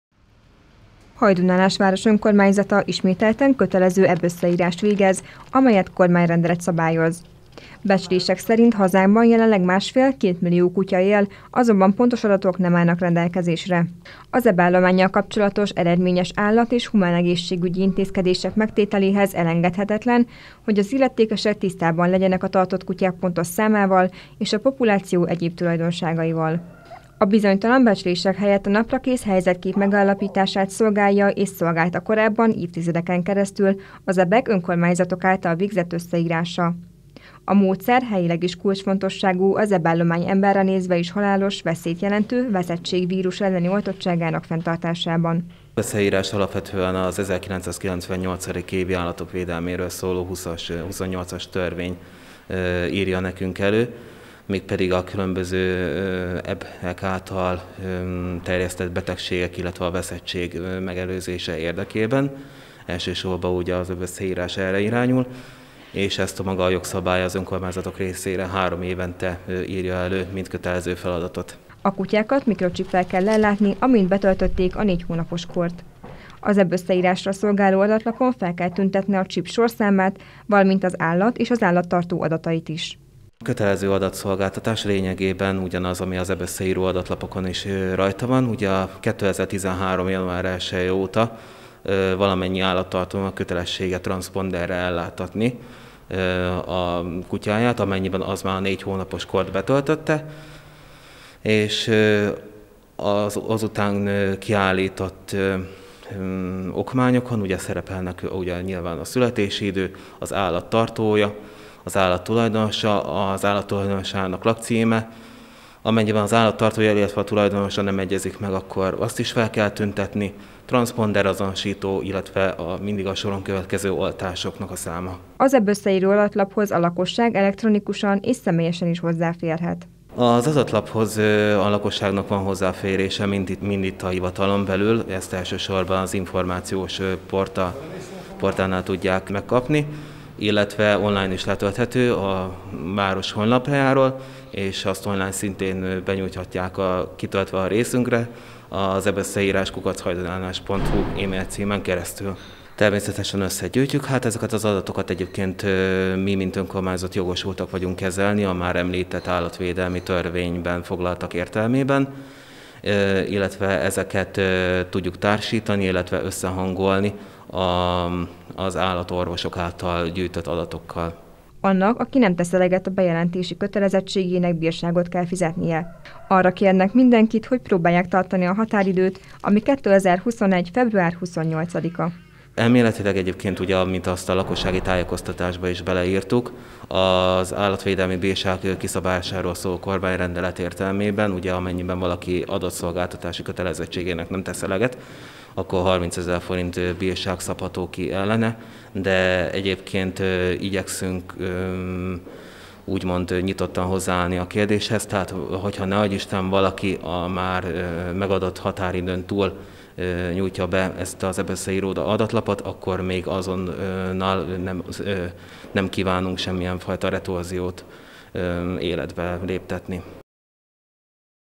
Hajdúnánási Televízió: Február végéig várják az adatszolgáltatást a kutyatartóktól 2021.02.12.
Idén ebösszeírást végez a helyi önkormányzat, amit három évente kormányrendelet alapján hajtanak végre. A kutyatartóknak kötelességük eleget tenni ennek az adatszolgáltatásnak. Amennyiben elmulasztják 30.000 Ft-os bírsággal sújthatók.